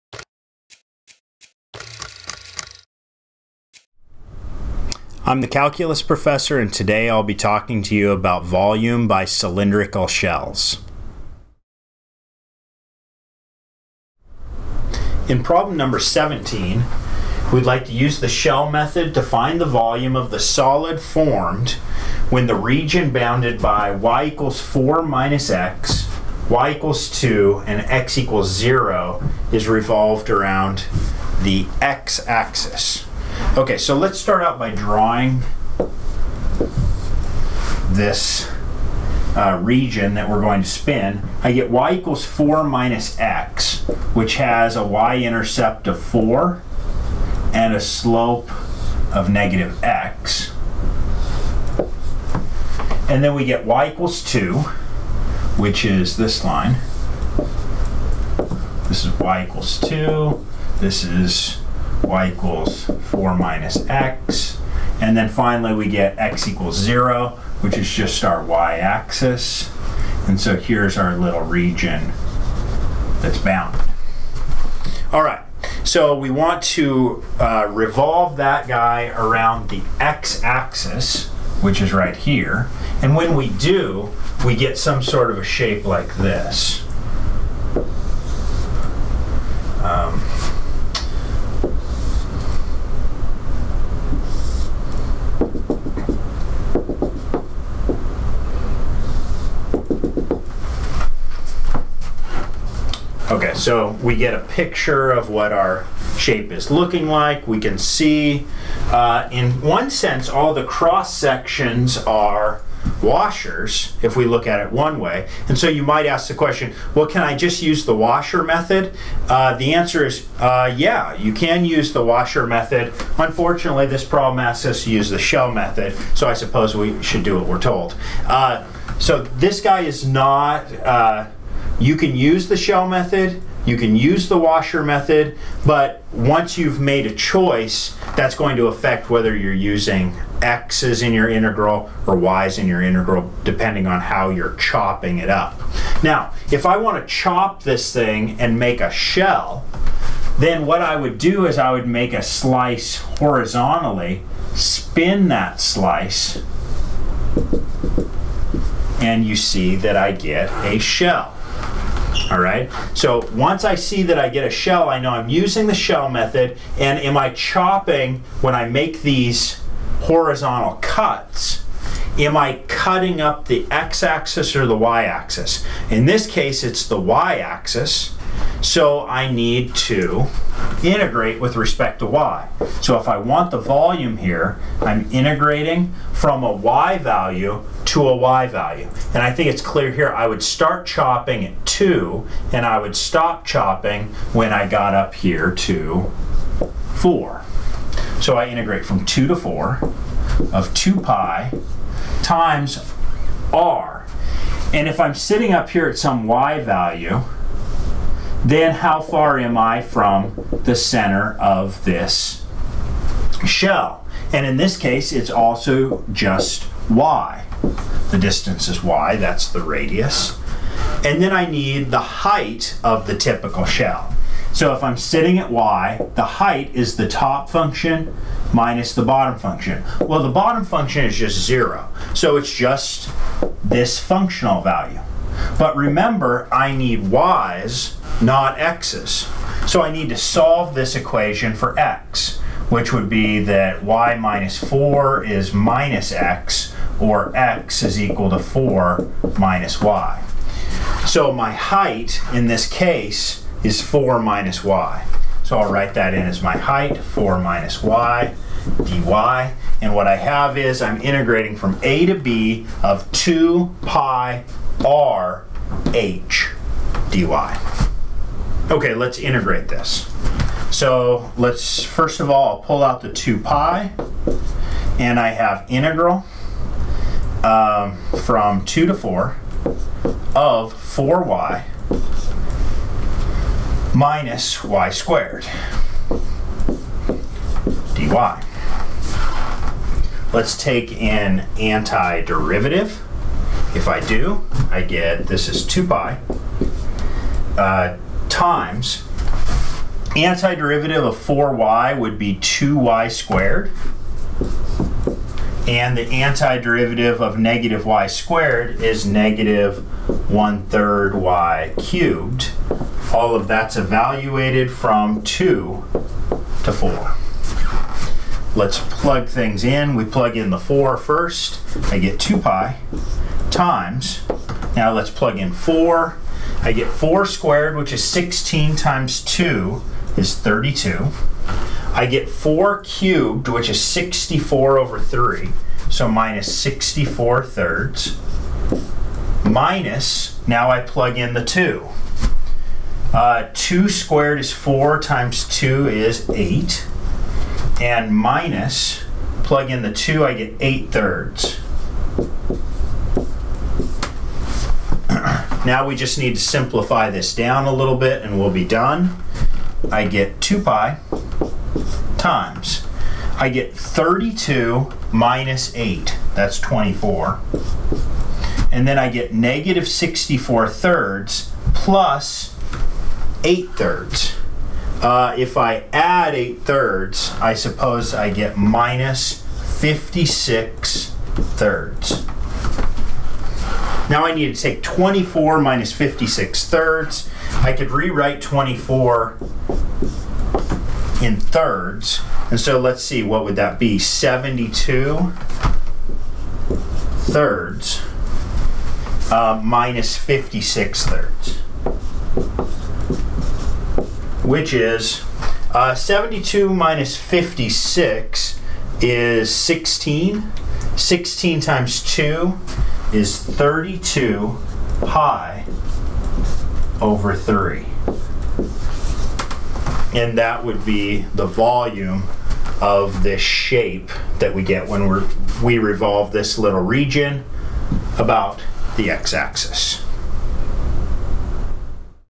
Volume by Shells, Example 5
Learn about finding volume in Calculus 2 using the method of cylindrical shells.

6.4 Volume by Shells # 17, page 443